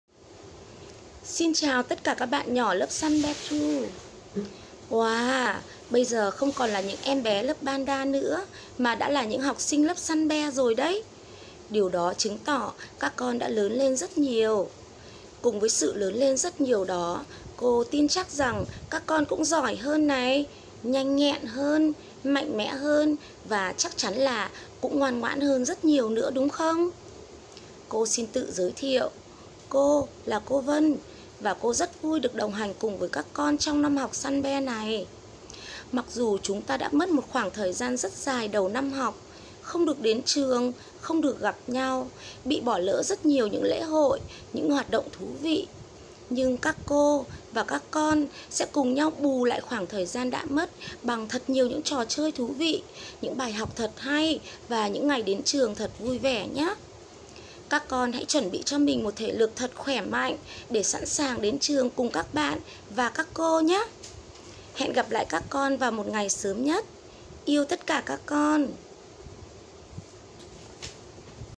Ms. Vân Zelda
Dear beloved students,
Welcome to an exceptional school year that promises to be a lot of fun.
Please listen to the greetings from the teachers who will accompany you in this school year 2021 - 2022.
Gửi các con học sinh thân yêu,
Chào mừng các con tới với một năm học thật đặc biệt và hứa hẹn thật nhiều niềm vui. Các con hãy cùng lắng nghe lời chia sẻ tới từ tập thể các thầy cô sẽ cùng đồng hành và gắn bó với chúng mình trong năm học này nhé.